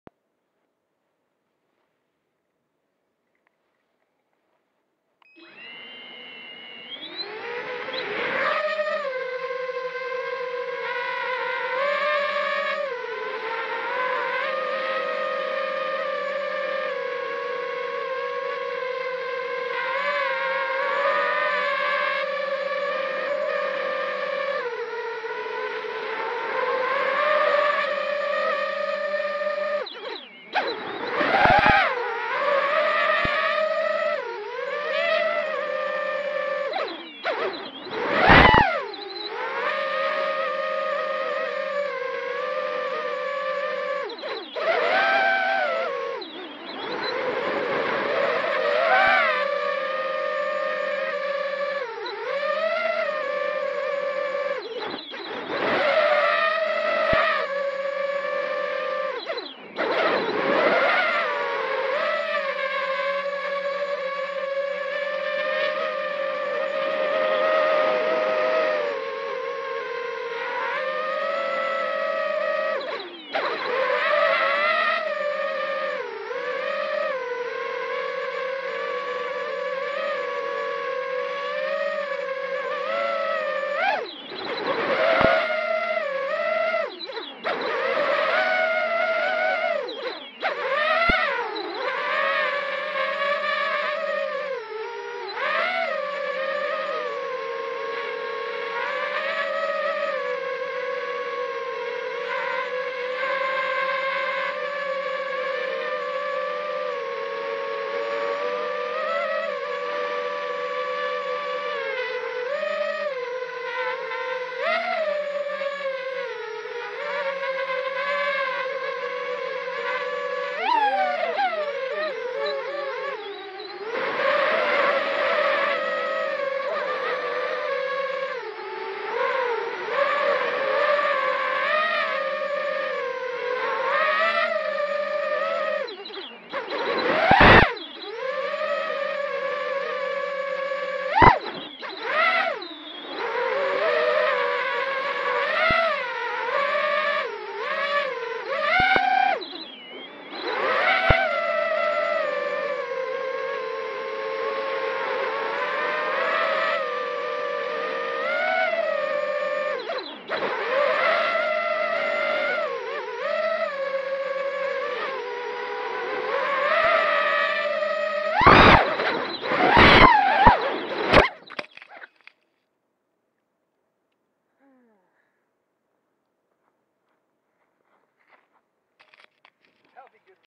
Flips in the Psycho quad
Attempting to do some flips in my micro quad called Psycho.  I had a runcam on top of my battery, which made it a bit unstable with all the weight on top like that.  Next time, I will put the runcam on the bottom to evenly distribute the weight.